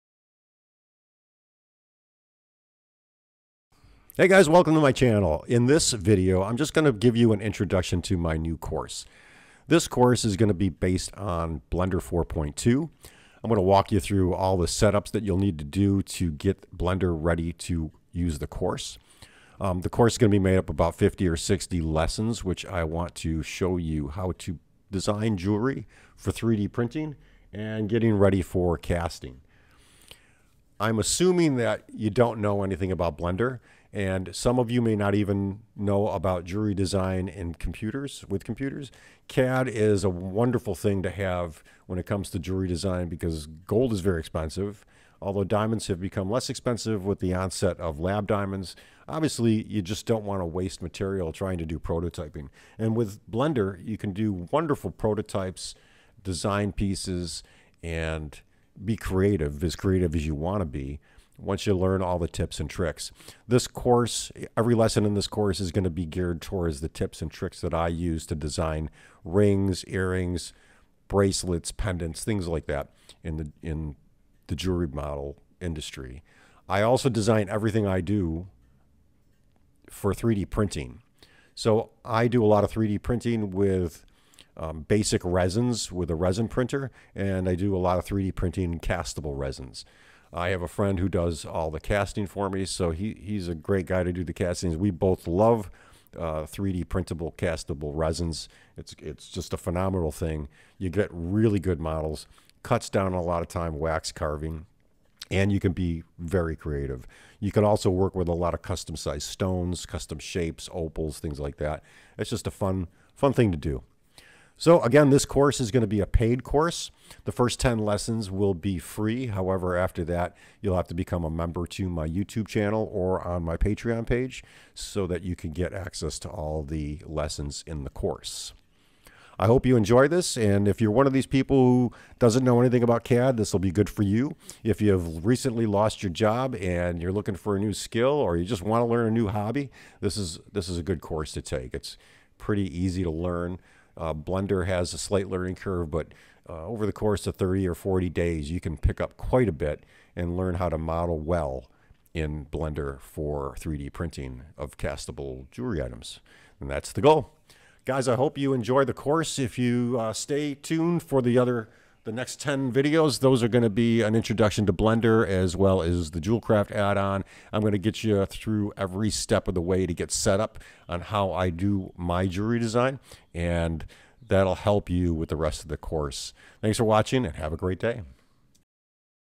Learn to make Jewelry with Blender 4.2 Introduction
This is an introduction to my new course... with this course, you will learn how to make jewelry with Blender 4.2 and a few free and paid addons for Blender.
This is a great way to improve your skills, add to your current product offerings, or maybe an entire new career.
I'm assuming that you don't know much about blender, or jewelry design with this course, so this is geared towards the beginner.
Lessons 1 thru 10 are free, but you will have to become a paid member of my channel to view the lessons after that...   If you're ready...   Lets Get Learning

Have a Question?  Need a watch Fixed?  Something Designed?